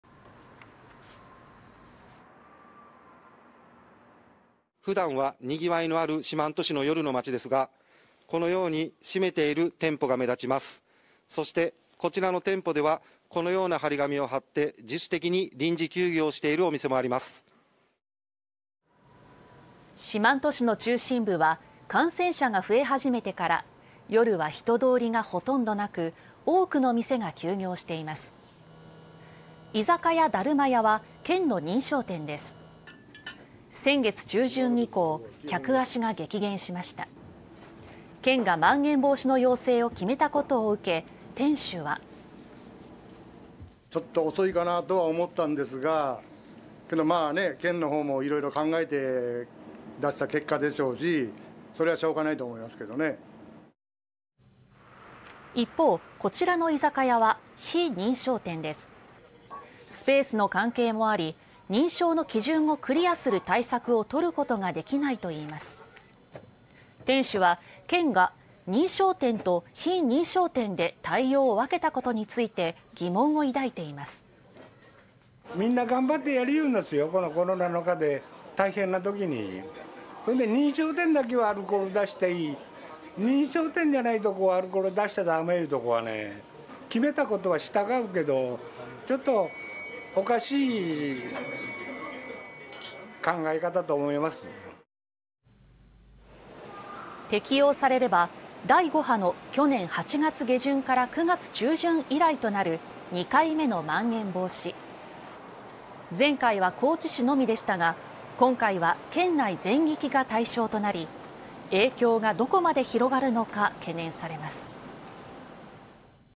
高知県「まん防」適用を国に要請 飲食店は・・・「ちょっと遅い」すでに自主休業の店も【高知】 (22/02/08 18:50)
適用されれば県内では2回目となる「まん延防止等重点措置」県内の飲食店から聞こえてきた声はー

【昨夜(7日）四万十市】
澤村栄治記者
「普段はにぎわいのある四万十市の夜の街ですが、閉めている店舗が目立つ。こちらの店舗ではこのような貼り紙を貼って自主的に臨時休業している店もあります。」

四万十市の中心部は感染者が増え始めてから夜は人通りがほとんどなく多くの店が休業しています。

居酒屋「達磨家」は県の認証店です。1月中旬以降、客足が激減しました。県が「まん延防止」の要請を決めたことを受け店主はー

達磨家・山本隆弘さん
「ちょっと遅いかなと思ったが、県もいろいろ考えた結果でしょうがないと思う」

一方、こちらの居酒屋は非認証店です。スペースの関係もあり、認証の基準をクリアする対策を取ることができないと言います。店主は県が認証店と非認証店で対応を分けたことについて疑問を抱いています。

なかひら・中平富士夫さん
「みんな頑張ってやっているコロナの中で大変なときに。認証店だけはアルコールを出していい、認証店でないところはアルコールを出しては駄目。決めたことは従うけどちょっとおかしい考え方。」

【去年8月・高知市】
適用されれば「第5波」だった去年8月下旬から9月中旬以来となる2回目の「まん延防止」。前回は高知市のみでしたが今回は県内全域が対象となり、影響がどこまで広がるのか懸念されます。